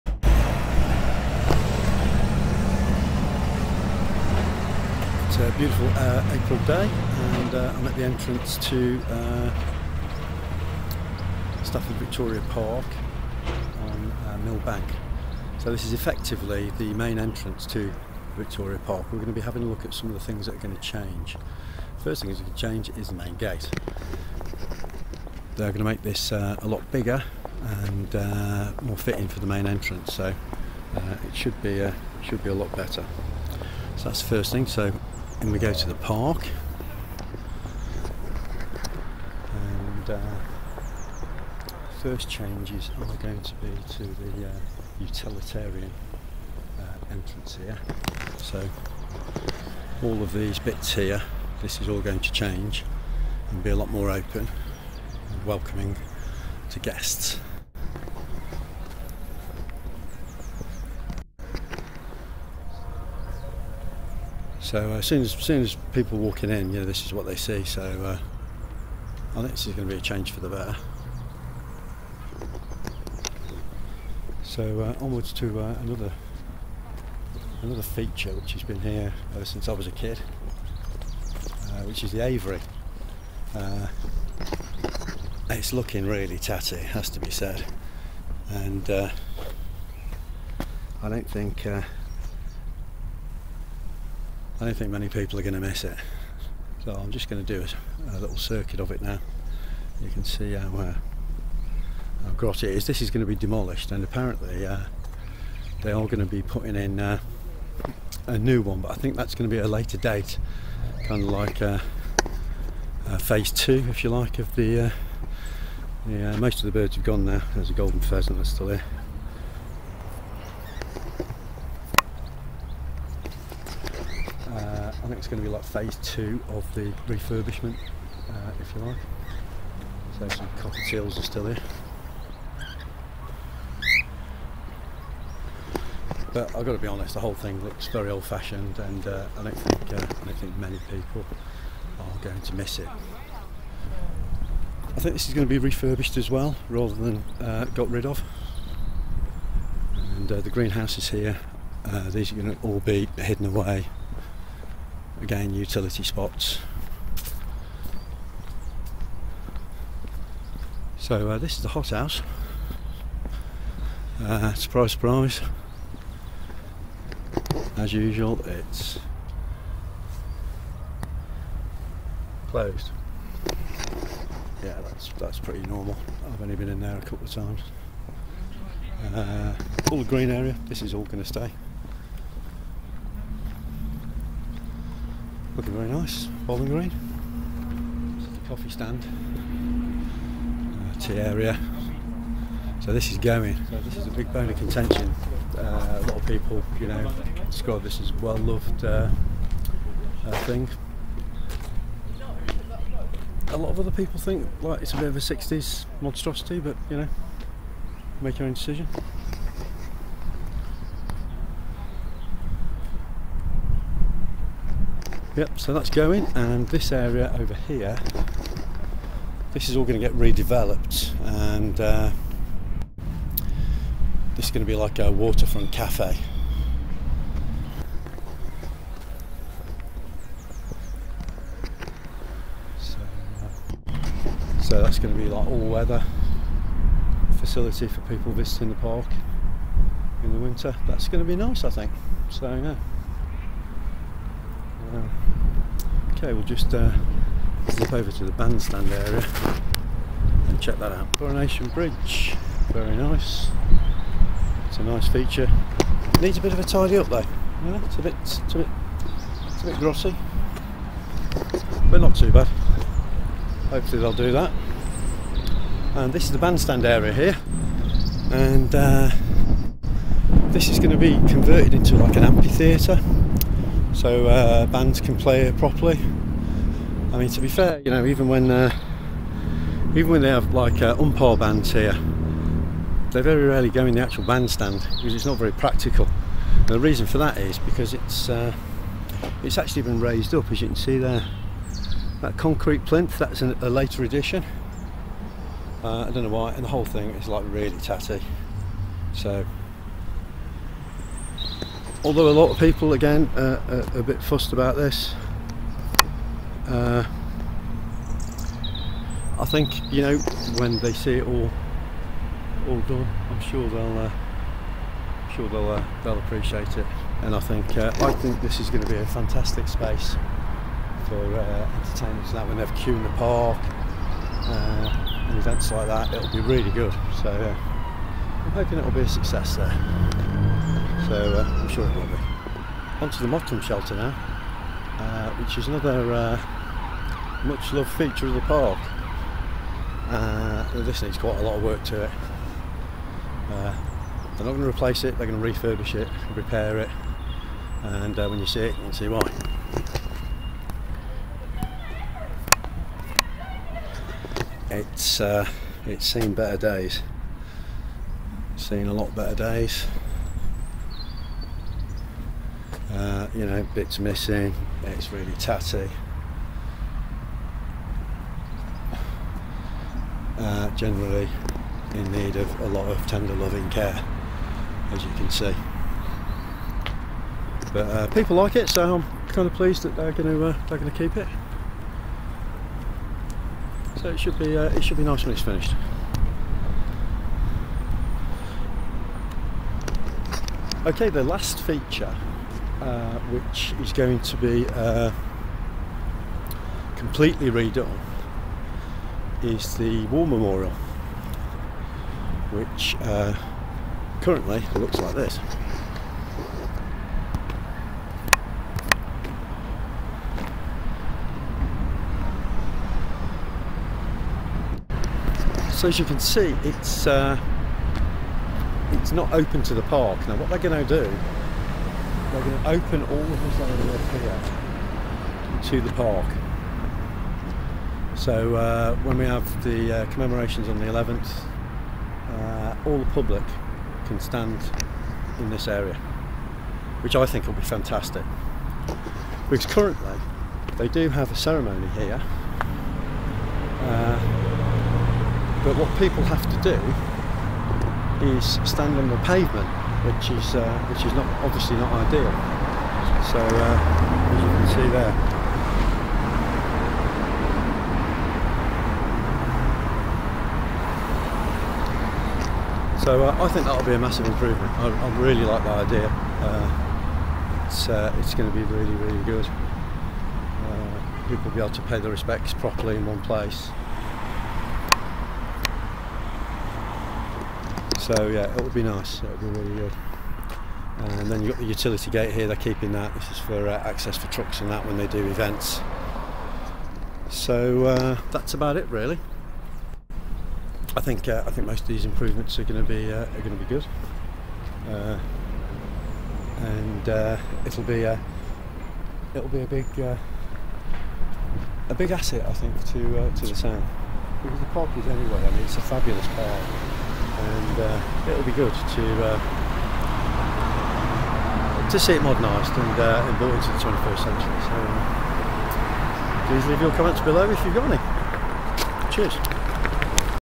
Victoria Park Stafford
A Video looking at the up-coming changes in Stafford's Victoria Park.